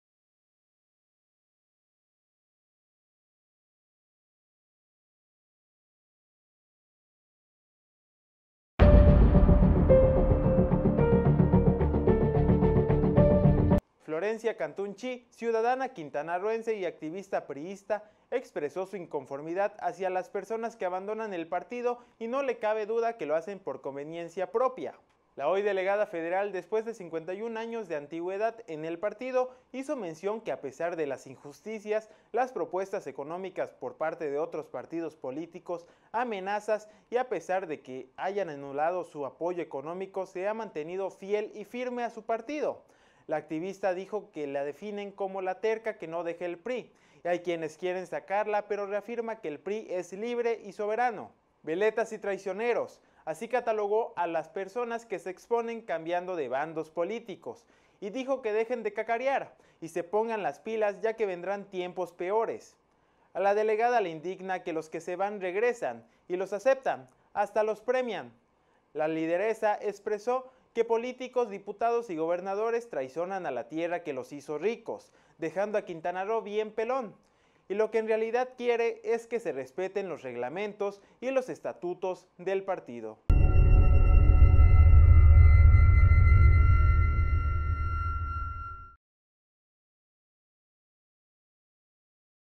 DE VELETAS CALIFICA ACTIVISTA DEL PRI A QUIENES ABANDONAN “EL BARCO”
Entérate de las noticias “EN CORTO”… DE VELETAS CALIFICA ACTIVISTA DEL PRI A QUIENES ABANDONAN “EL BARCO”…